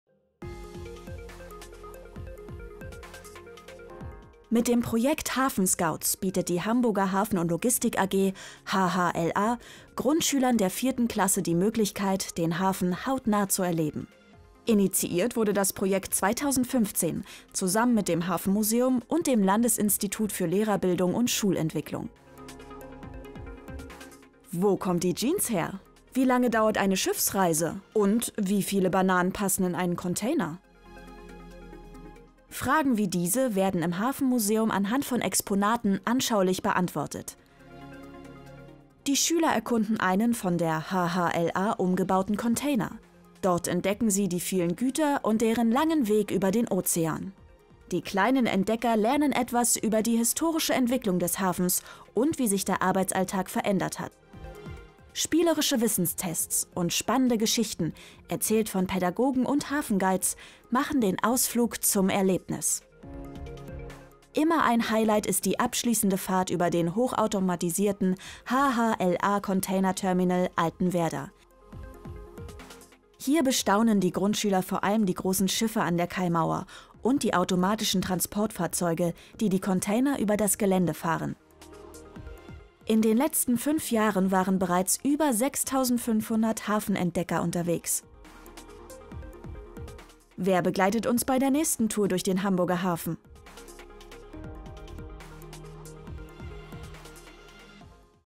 HHLA HafenScouts – Unterwegs im Hamburg Hafen | Gesellschaftliches Engagement
Maritime Logistik hautnah erleben! Grundschüler der Klasse 4 erkunden als Hafen-Scouts maritime Logistik im Hafenmuseum Hamburg und auf dem ersten klimaneutralen Container Terminal Altenwerder. Einblicke in Hafen damals und moderne Logistikkonzepte heute wie selbstfahrende AGVs und automatisierte Blocklager.